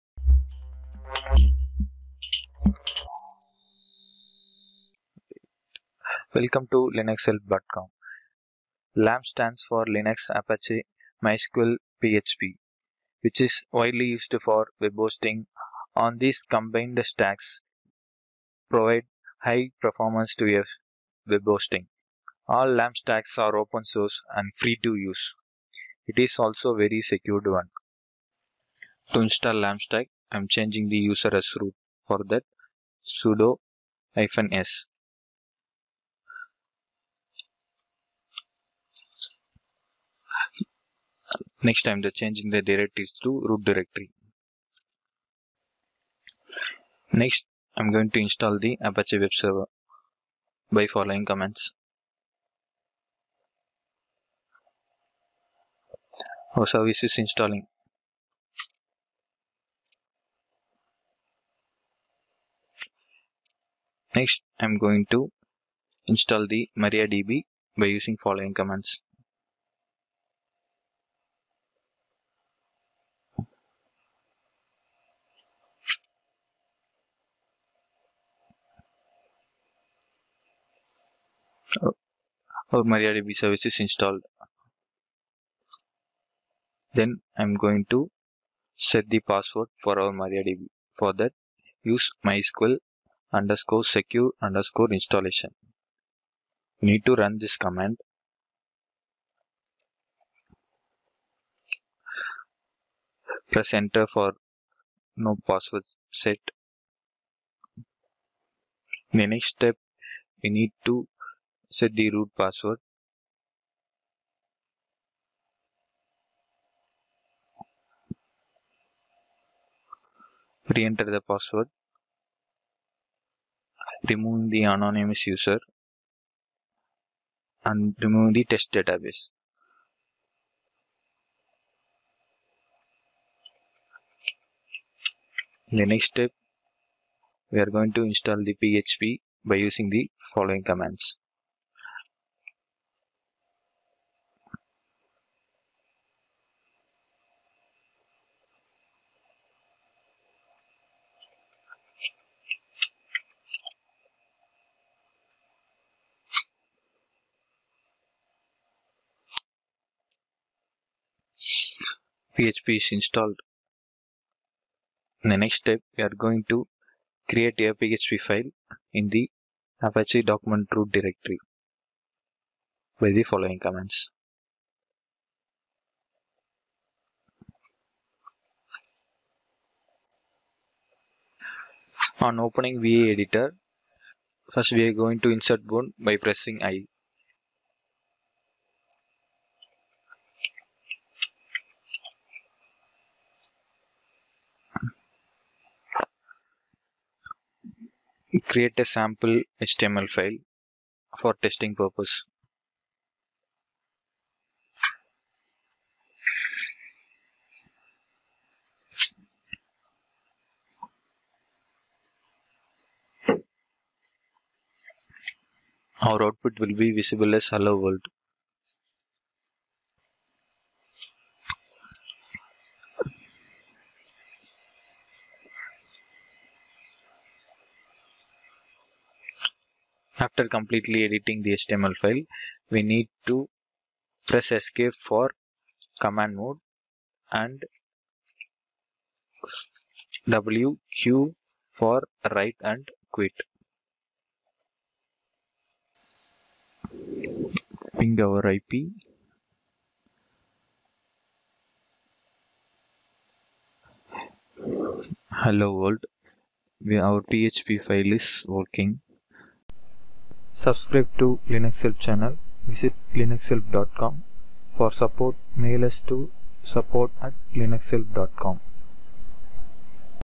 How to install LAMP setup on Ubuntu 21
Know about this prominent open source web platform that aims in providing higher performance for web applications.